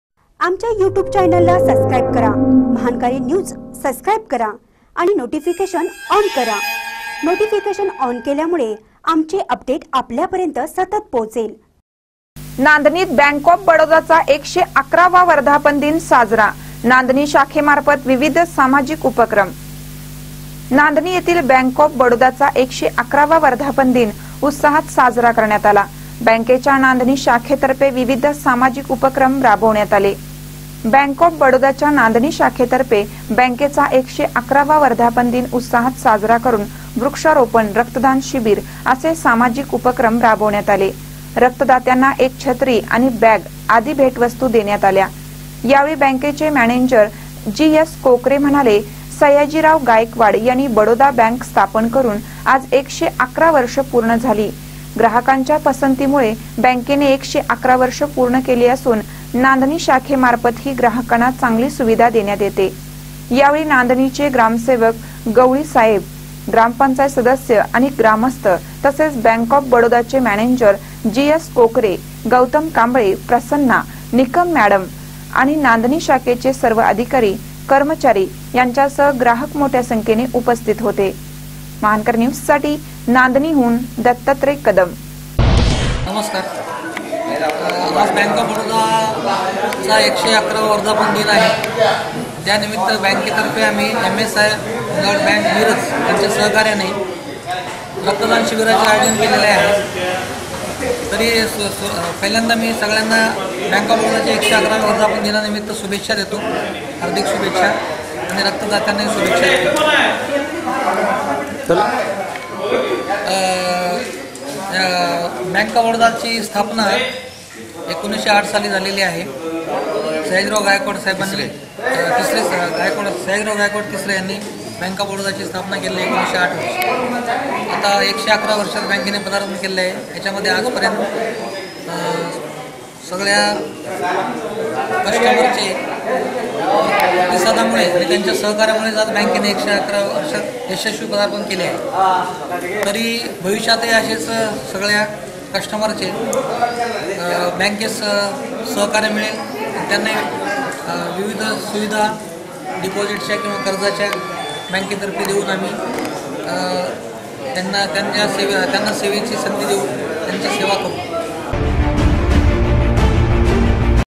नांदणीत बँक ऑफ बडोदाचा १११ वा वर्धापन दिन साजरा
नांदणीत बँक ऑफ बडोदाचा १११ वा वर्धापन दिन साजरा : नांदणी शाखेमार्फत विविध सामाजिक उपक्रम